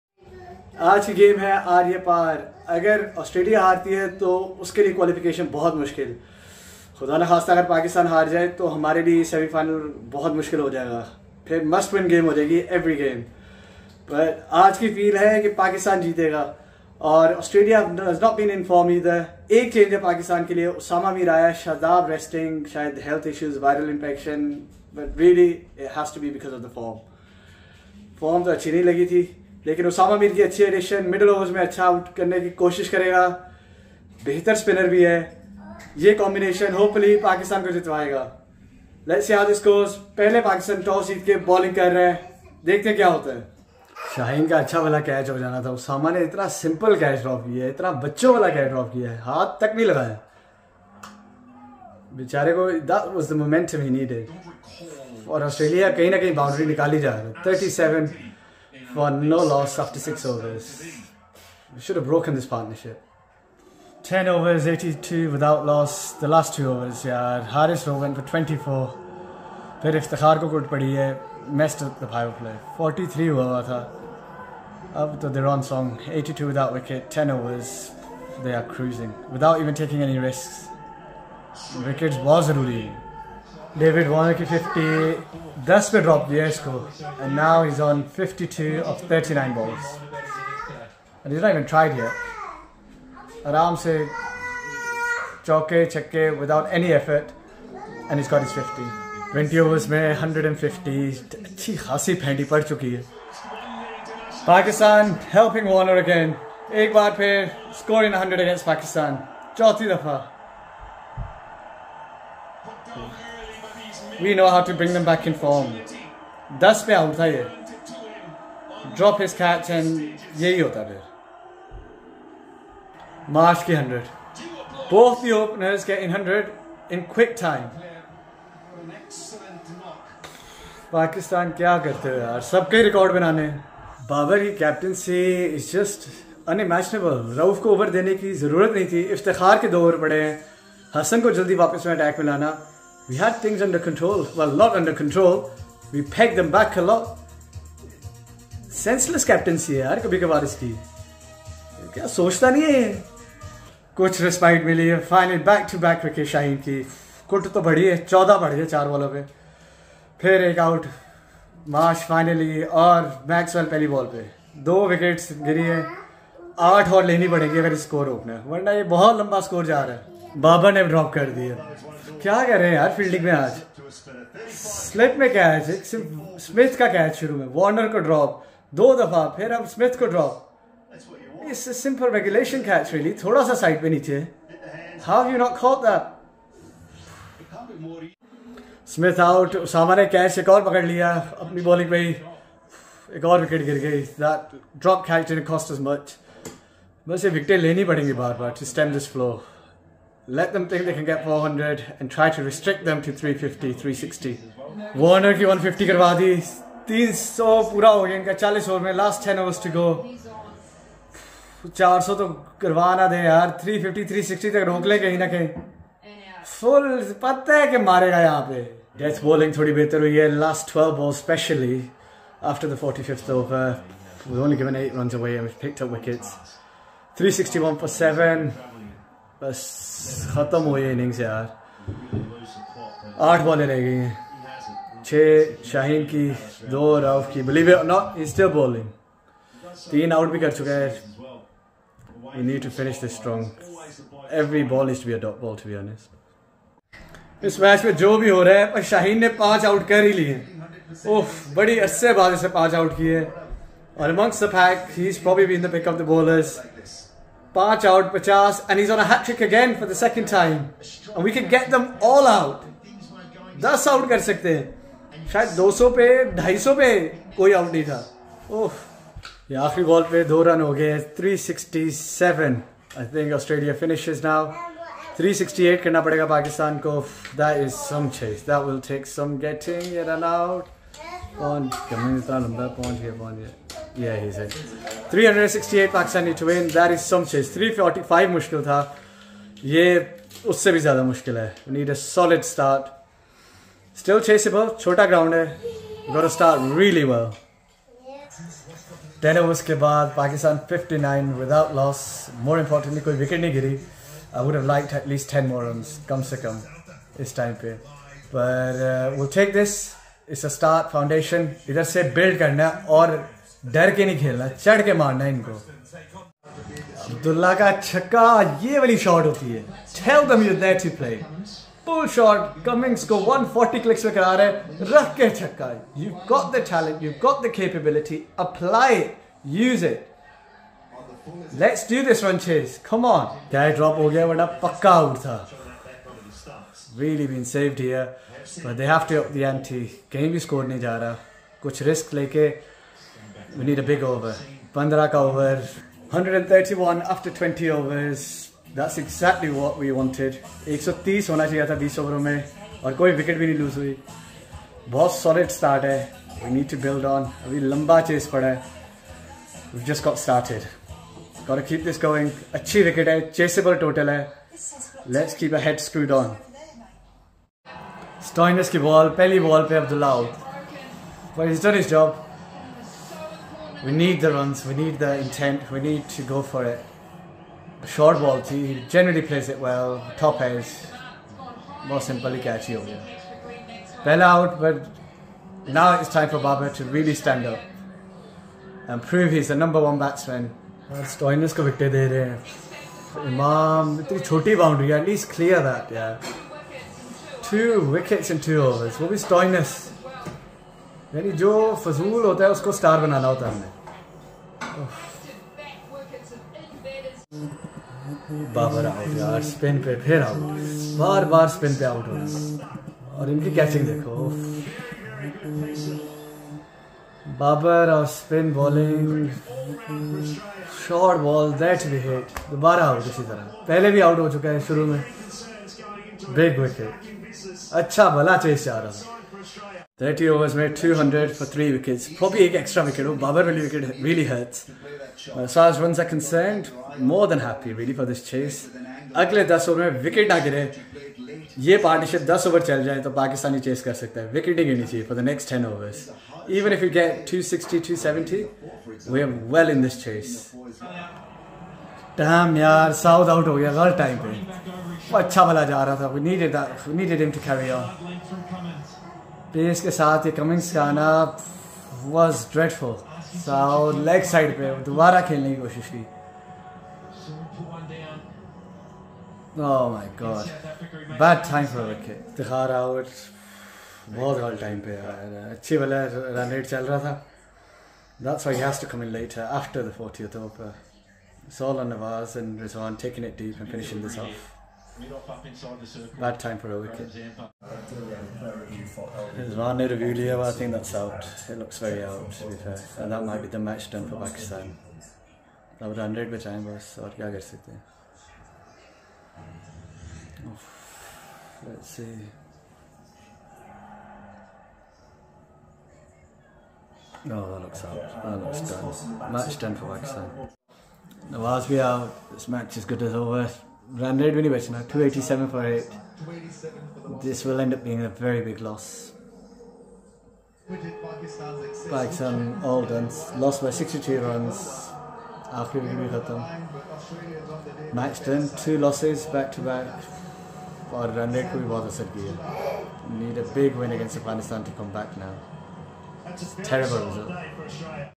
PAKvAUS VLOG| DREADFUL CAPTAINCY| DROP CATCHES AND AVERAGE BOWLING| AUS ALWAYS A PAIN FOR PAK
Another loss and poor show continues from Pakistan Team. Looked all over the place and fans deserve better!